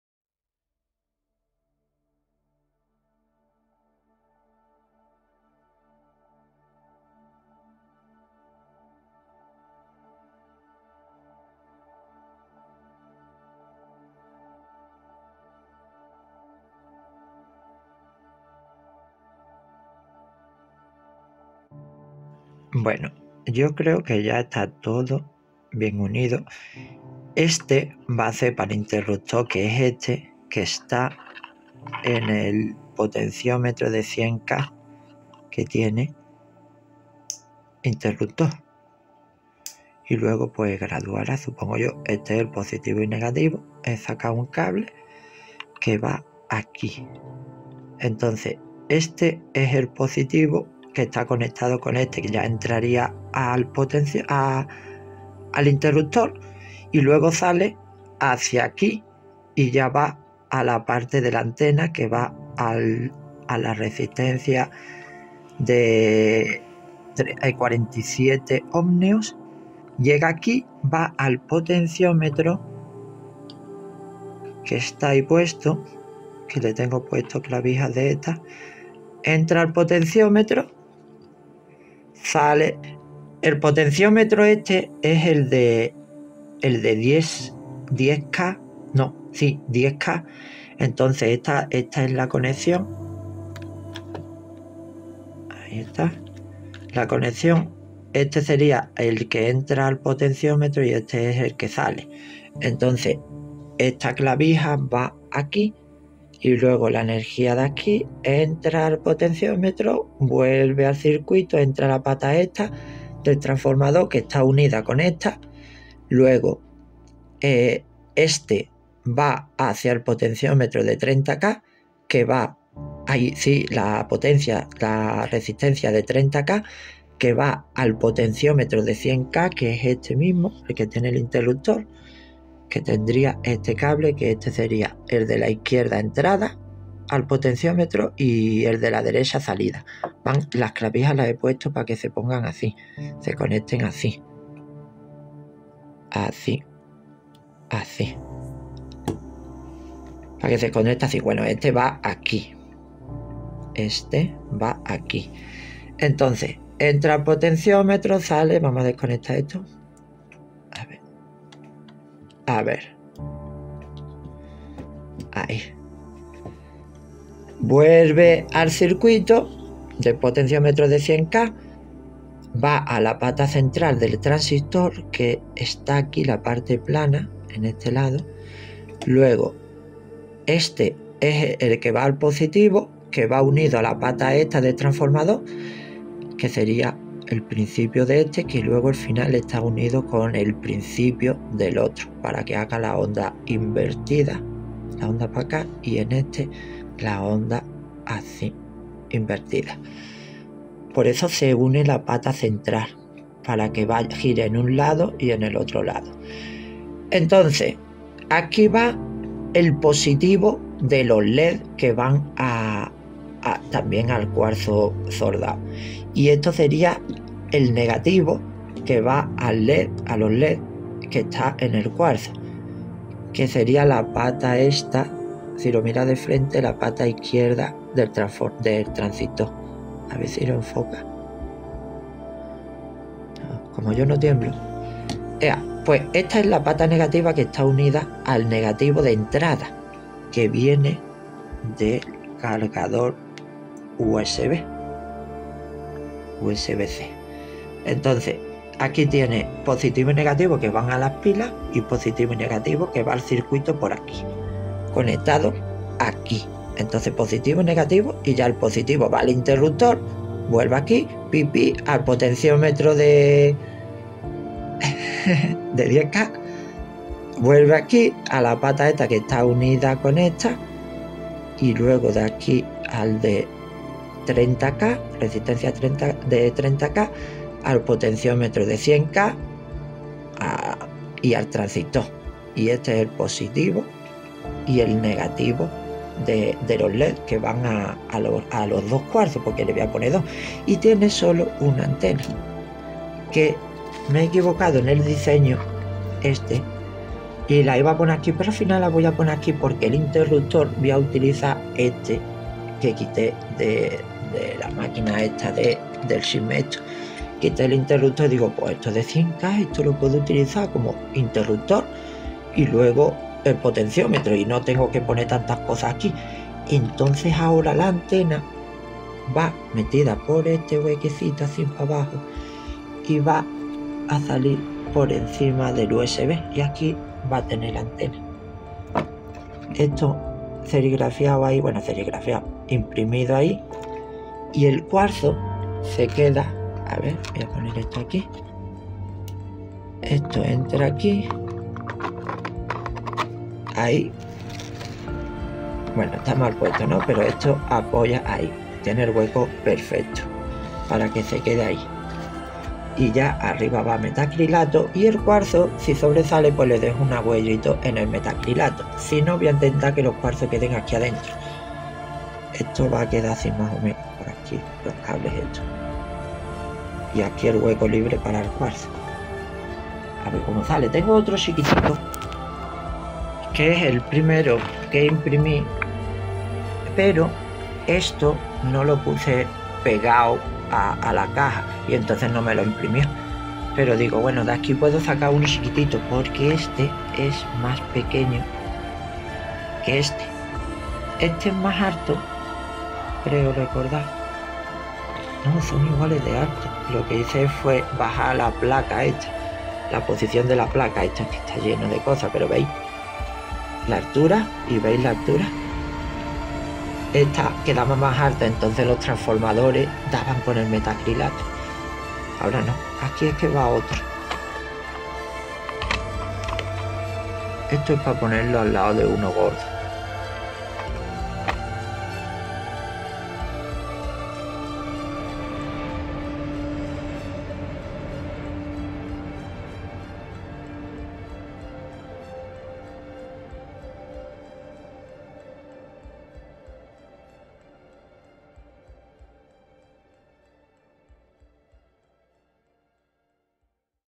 El Amplificador de Orgon OA2000 de 3.7V
Voy a hacer 4 más de estos, pero voy a modificar el hueco donde va a ir la antena, ya se donde ponerla creo jajaja.